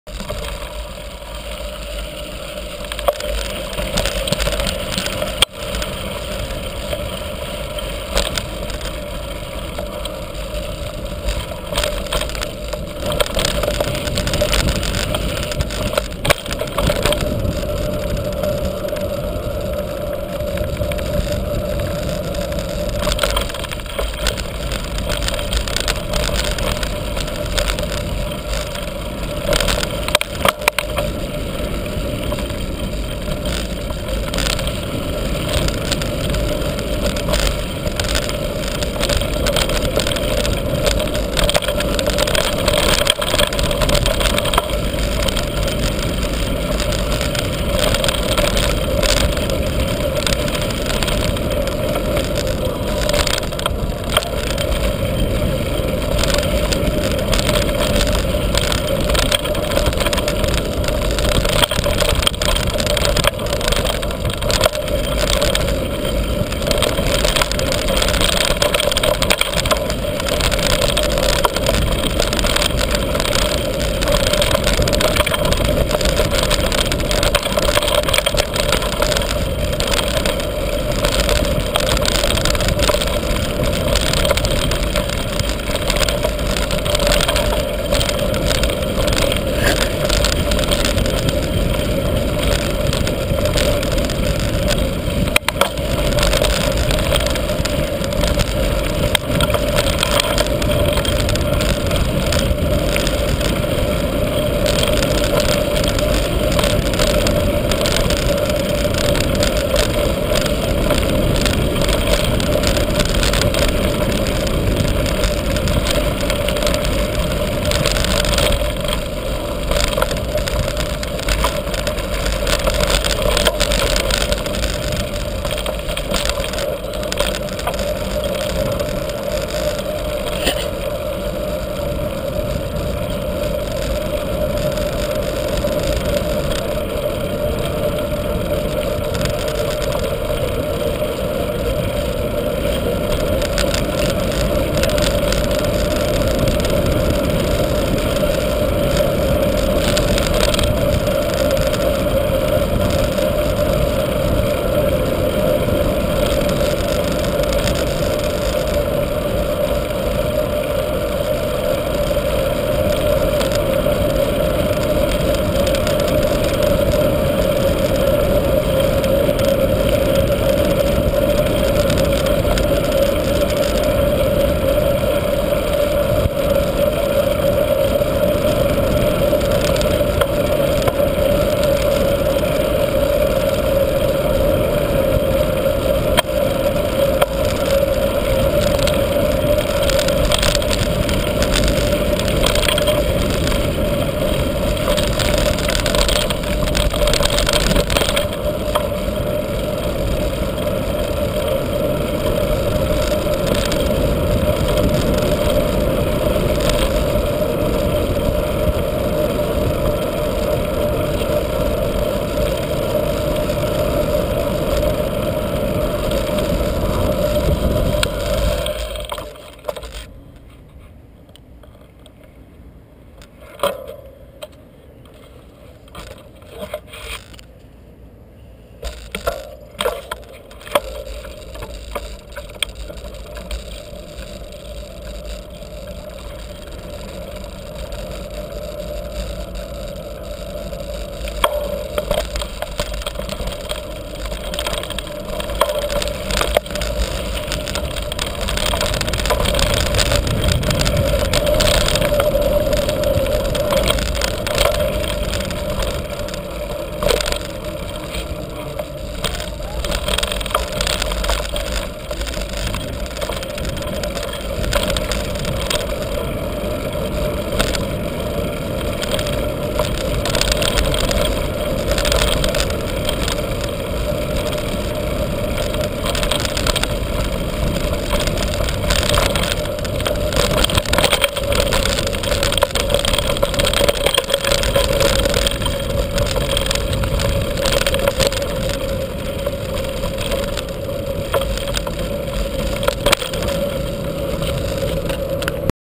Full downhill at "Cerro Garabitas" in Casa de Campo
Full downhill at Cerro Garabitas by alternative ways, very fast and technical.